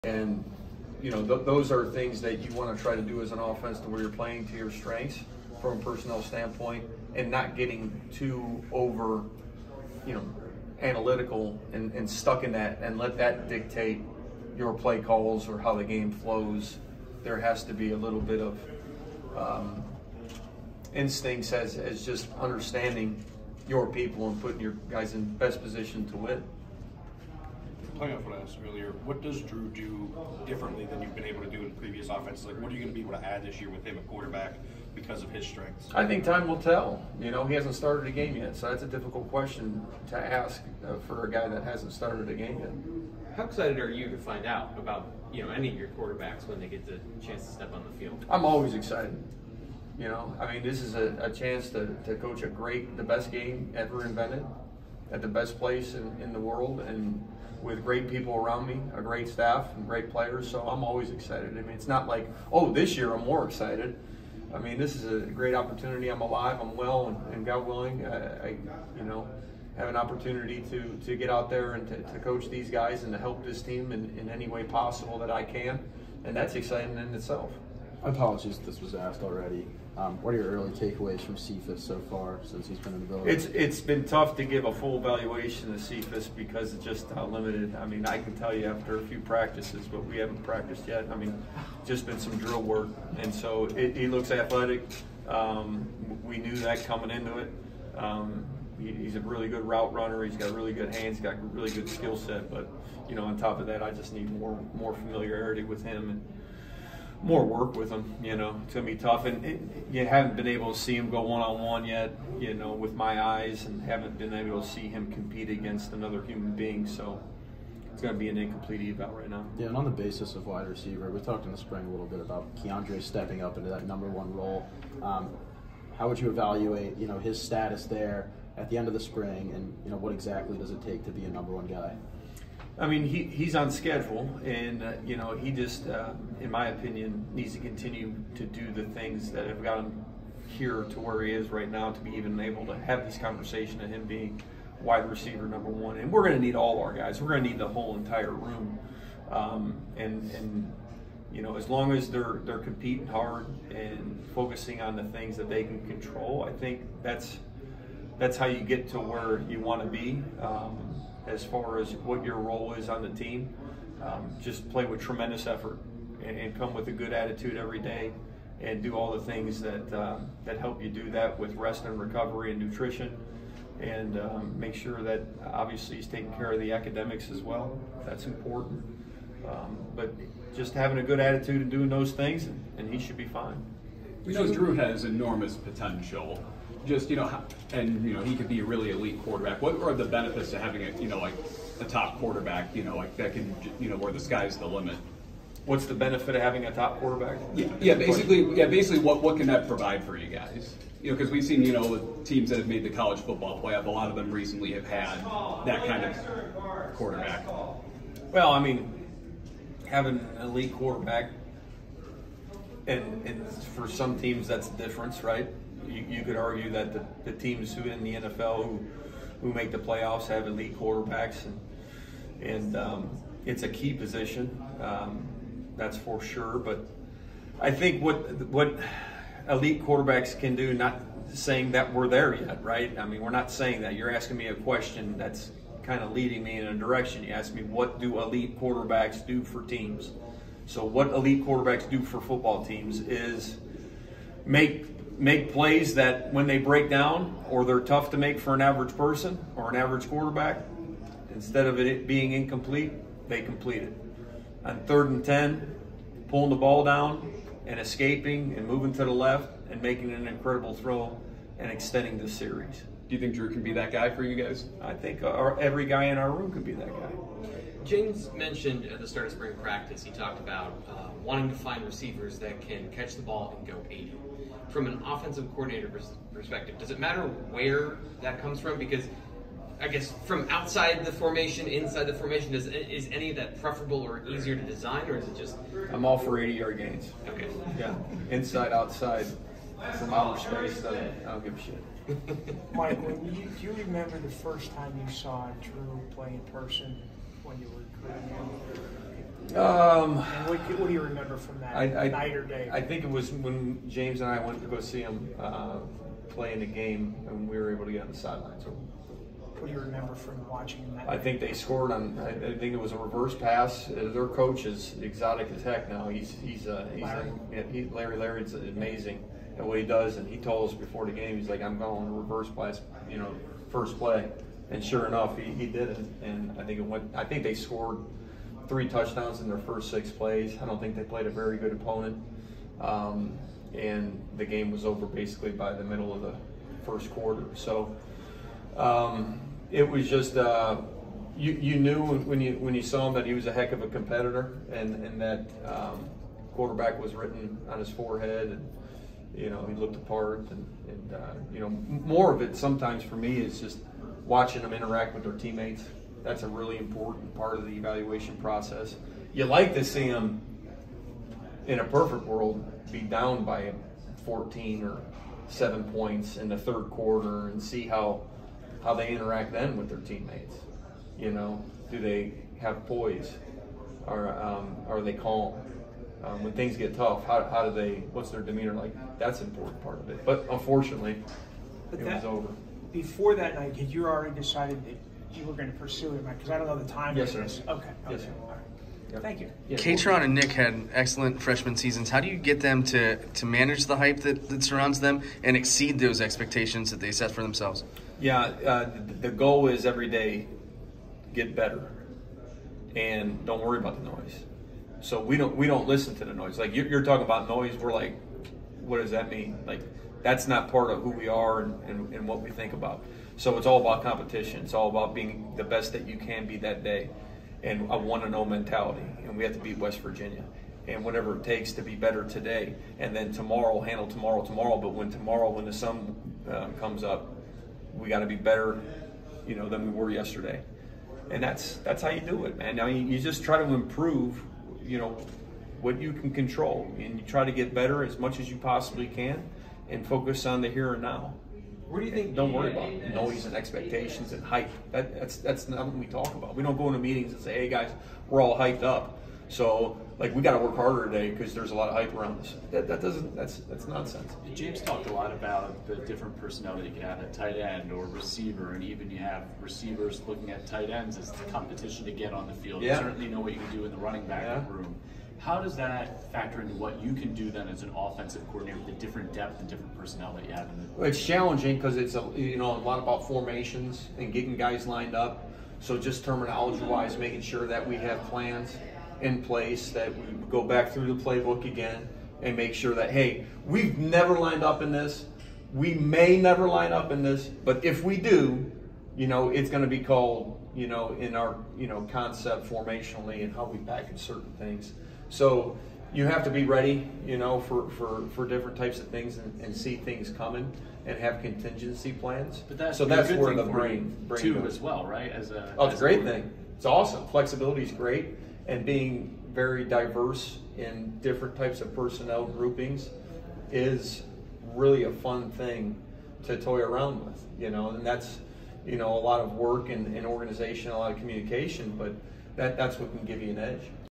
PSU OC Mike Yurcich - 6/8/2023 | Nittany Sports Now
Penn State offensive coordinator Mike Yurcich speaks with the media on June 8, 2023